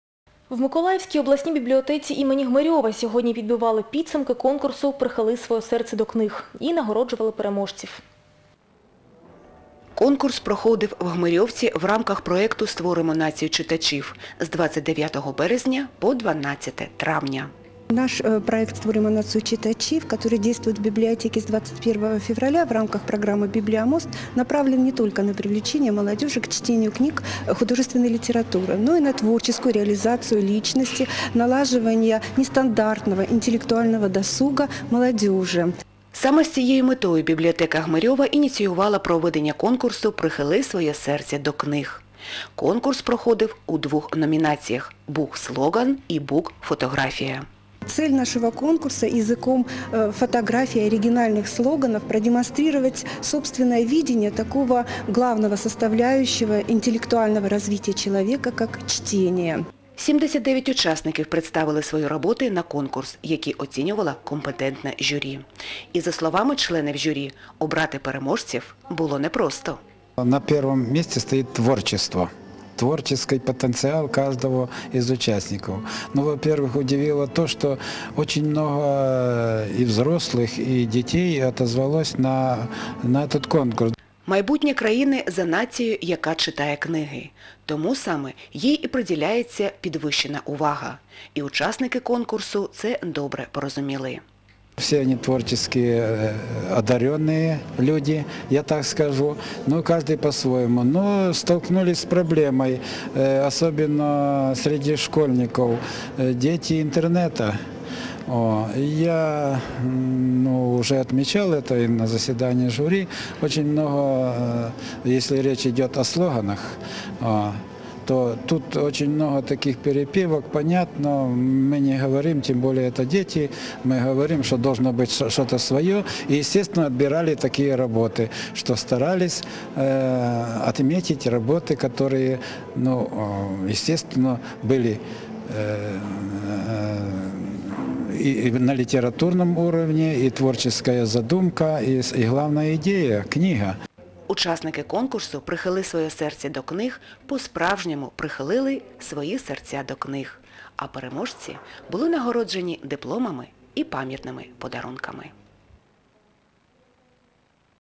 Hmyryov_library_Bibliomist_project_on_regional_TV.mpeg
Mykolayiv oblast TV channel "Mykolayiv TV: TNN" program on results of "Get Loyal to Books" competition held at Hmyryov oblast library as part of Bibliomist project (aired on May 18, 2011).